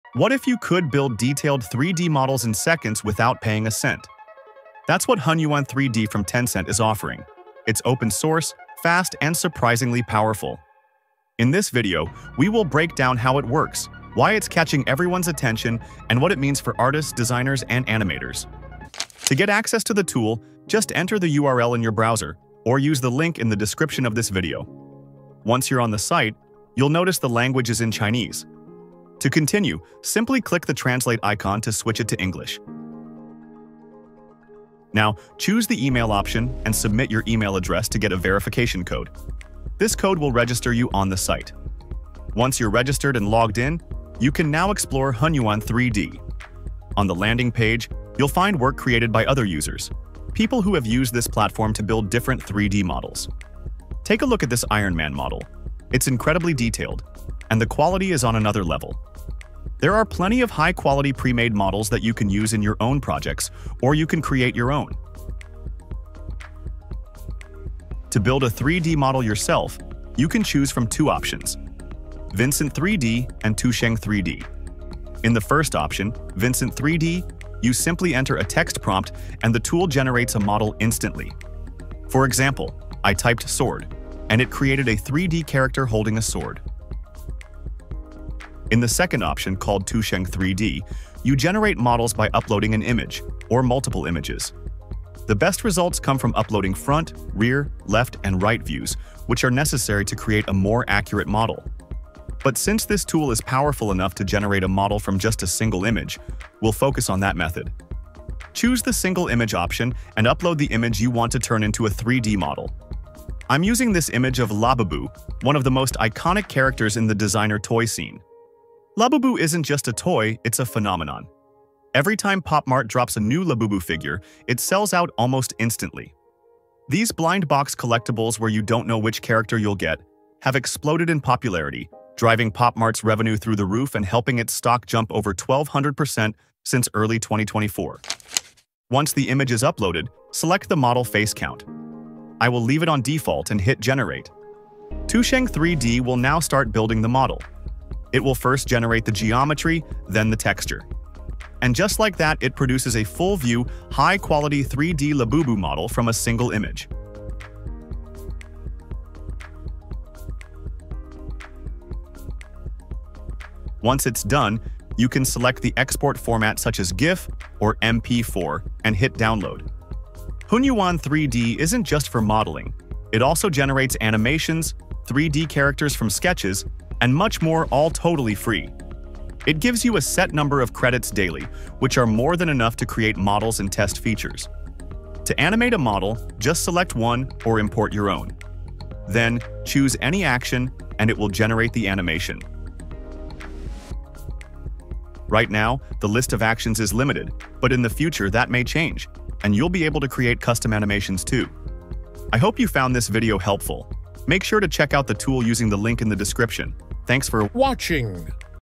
Convert 2D images to 3D models with AI for free | Hunyuan 3D-2.5
Looking for the best way to convert your 2D images to 3D models using AI? In this video, we explore Tencent Hunyuan 3D-2.5, a powerful and free AI tool that makes 3D modeling easier than ever! Whether you're a designer, developer, or enthusiast, this AI tool for 3D modeling lets you generate stunning 3D AI models directly from flat 2D images.

We'll show you step-by-step how to convert 2D to 3D with AI for free, covering the entire process of using this 3D AI model generator free. From beginners to pros, anyone can now generate 3D objects using AI for free without needing expensive software.

✅ Topics Covered:

How to use AI tool to turn 2D to 3D AI

The easiest way to try an image to 3D model AI

Review of Tencent Hunyuan 3D-2.5 features

Real-time demo of this free AI tool for 3D conversion

What makes this 2D to 3D model AI free tool stand out

👍Learn Graphic design from scratch to advance: The best course which helped me: